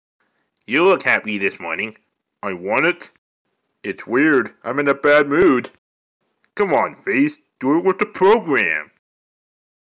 Google Translated Garfield Comic Fandub
Square Root of Minus Garfield comic by AQ206.

This is one of the Google Translated Garfield comics I thought of reading.